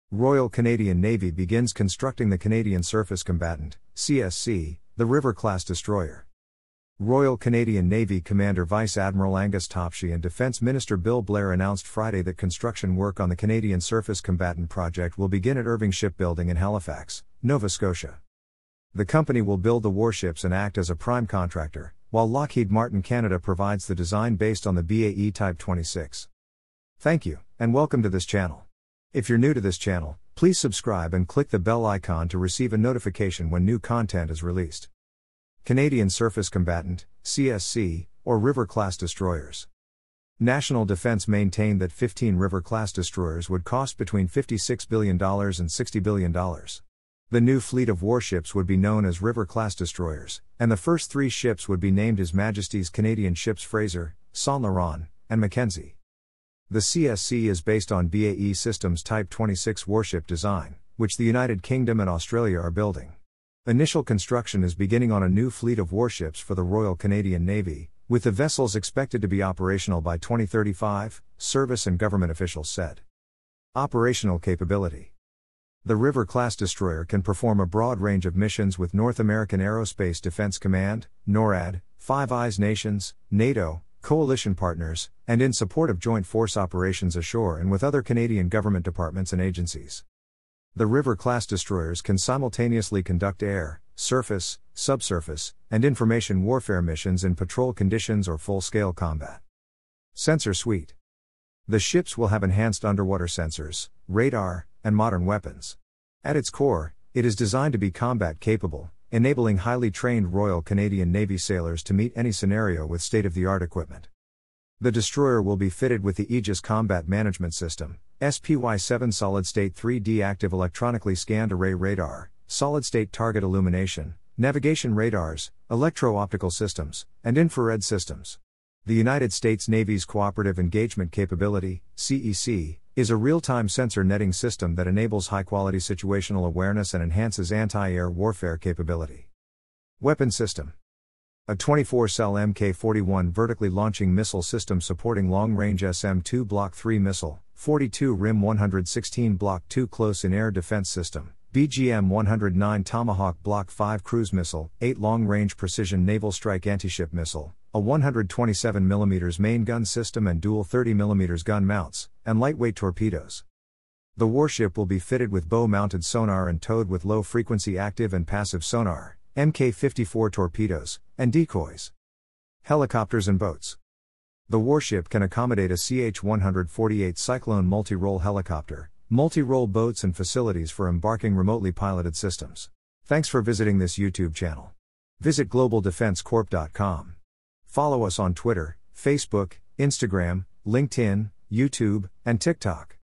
ROYAL CANADIAN NAVY BEGINS CONSTRUCTING CANADIAN SURFACE COMBATANT (CSC), RIVER-CLASS DESTROYER.
The CSC is based on BAE Systems Type 26 warship design, which the United Kingdom and Australia are building. Initial construction is beginning on a new fleet of warships for the Royal Canadian Navy, with the vessels expected to be operational by 2035, service and government officials said.